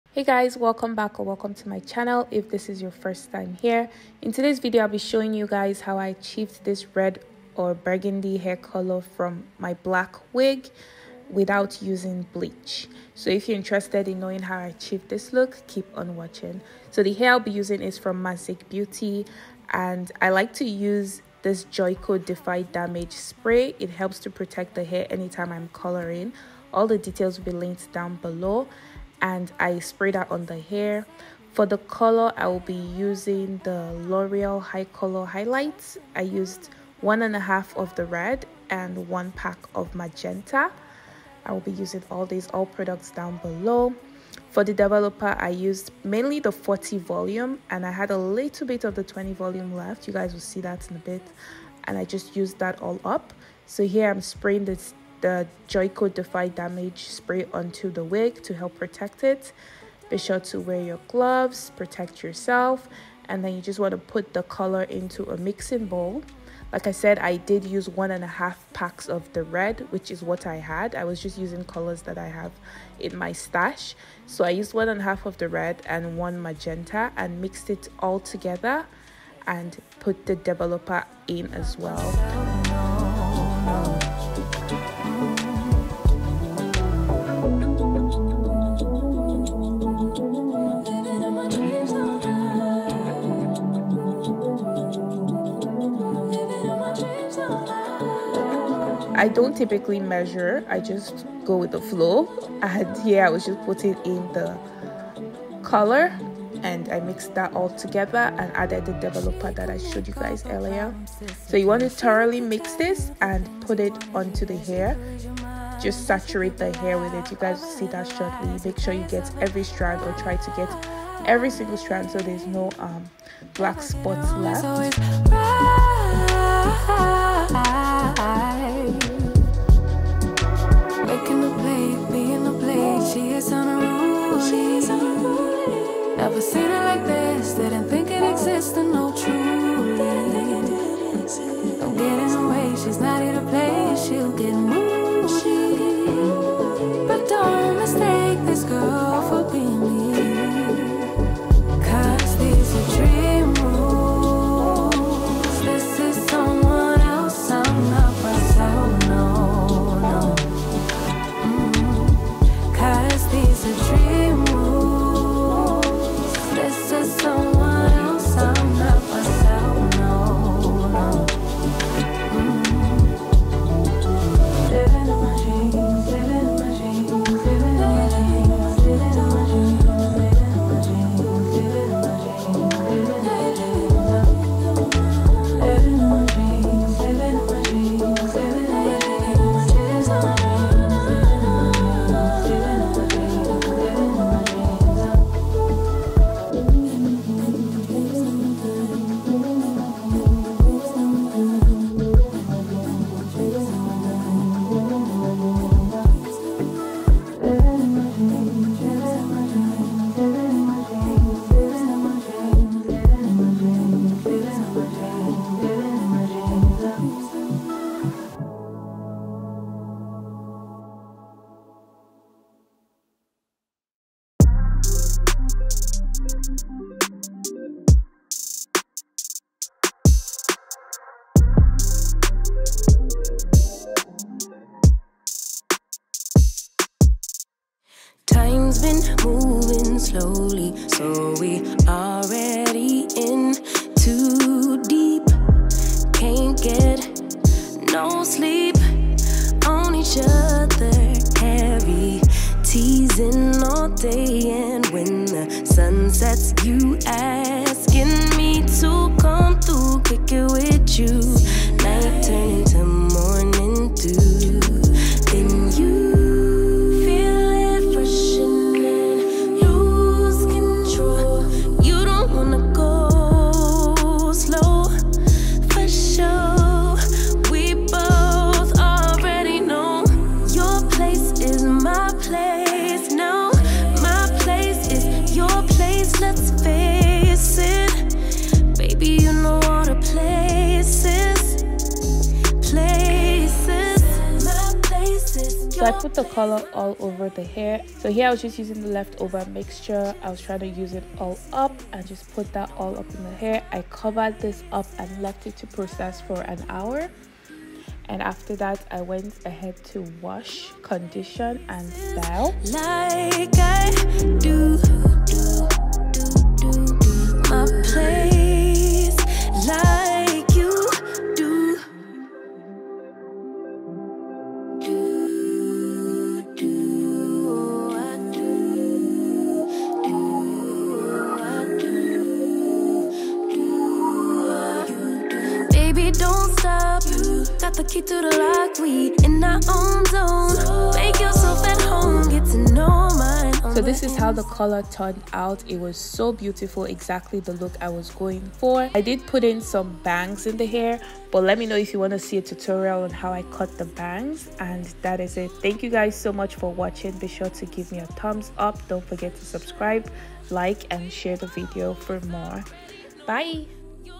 How To: Dye Your Hair From Black To Red/Burgundy Without Using Bleach | Mazic Beauty Hair
Hey friends! This video is on how I dyed my Mazic Beauty wig from black to red/burgundy without using bleach.

Products:
Joico defy damage spray
L'oreal hi color highlights in red
L'oreal hi color highlights in magenta
20 vol salon care cream developer

*I have no rights to music used

Cheers
M.